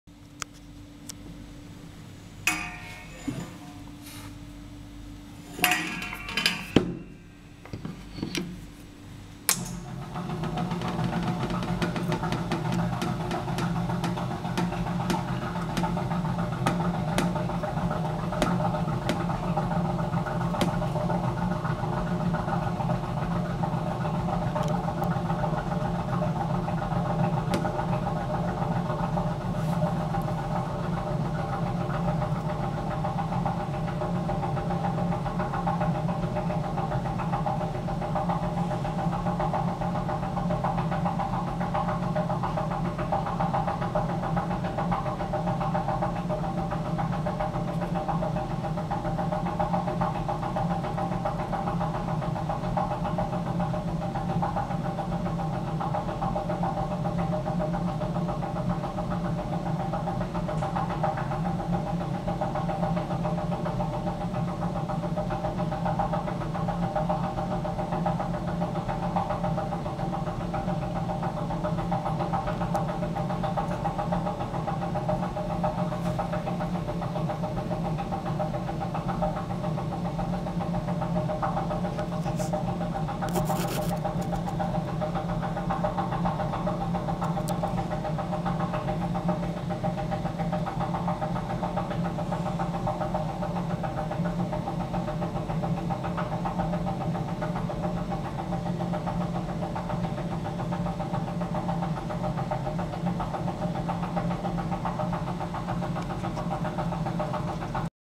Pumping the liquid nitrogen to get some solid nitrogen.
Supercool liquid.